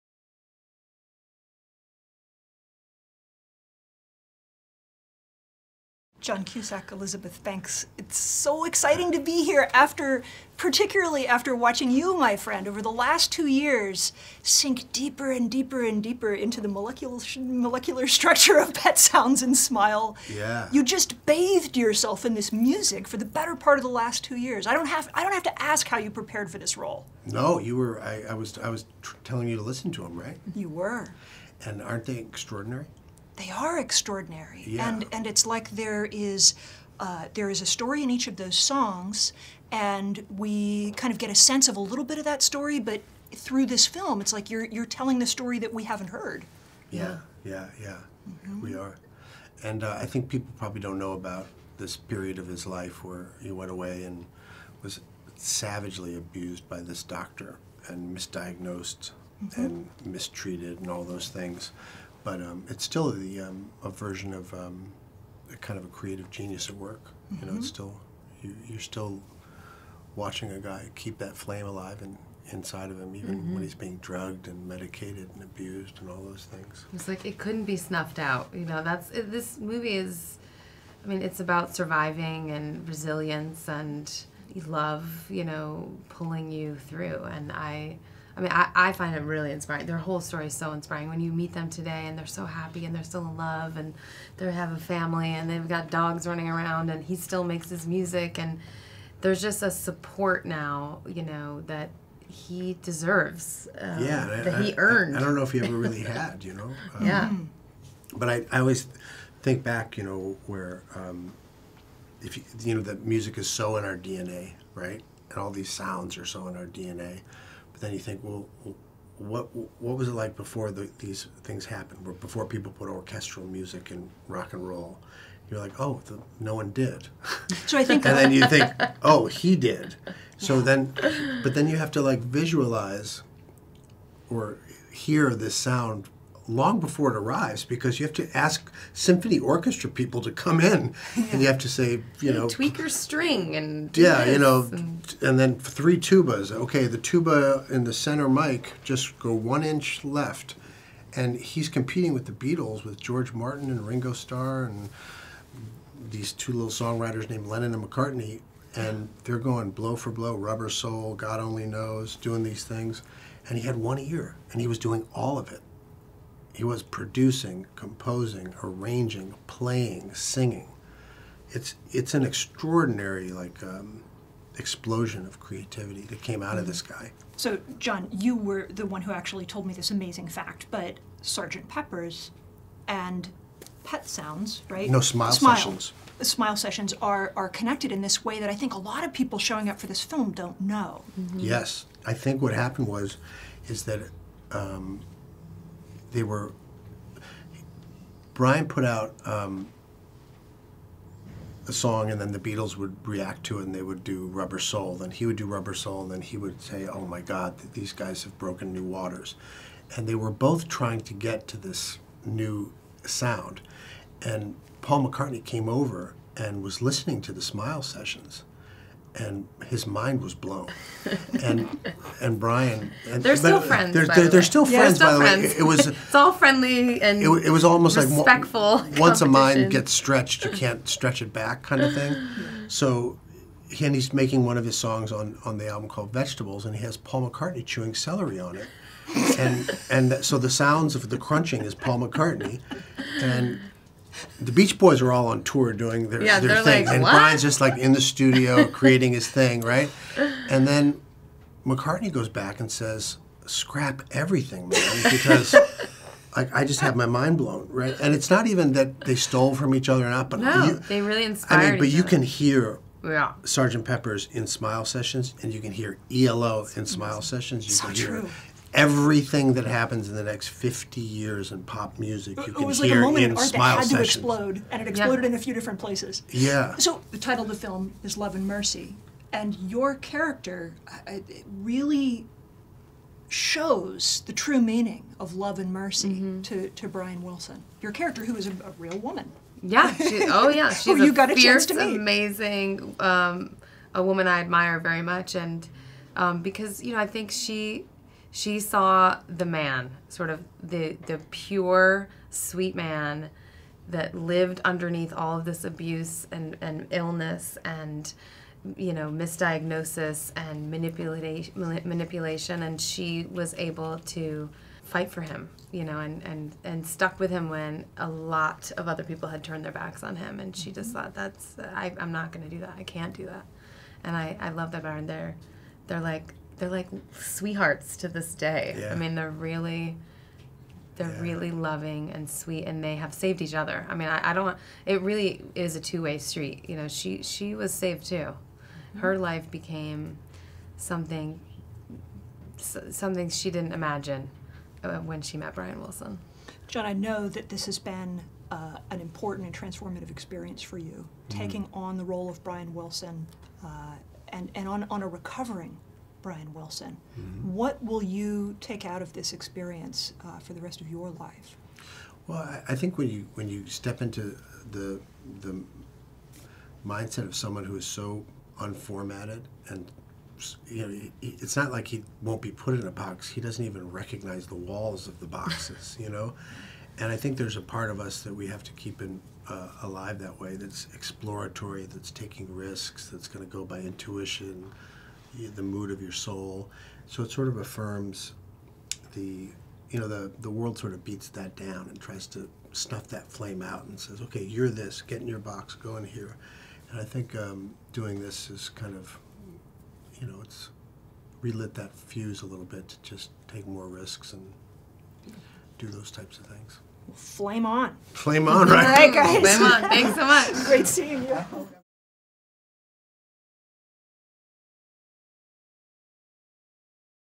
Love & Mercy: John Cusack and Elizabeth Banks on the life story of Brian Wilson
Boing Boing's Xeni Jardin speaks with John Cusack and Elizabeth Banks, the stars of “Love and Mercy,” a film about the life, love, and musical genius of the Beach Boys' Brian Wilson. The movie stars Paul Dano as a young Wilson, John Cusack as an older Wilson navigating mental illness and surviving a history of family alcoholism and abuse. Elizabeth Banks plays Melinda Ledbetter, who helps the older Wilson escape from an abusive relationship with a controlling therapist played by Paul Giamatti.

Directed by Bill Pohlad with a score by Atticus Ross, “Love and Mercy” opens in theaters around the US on Friday, June 5, 2015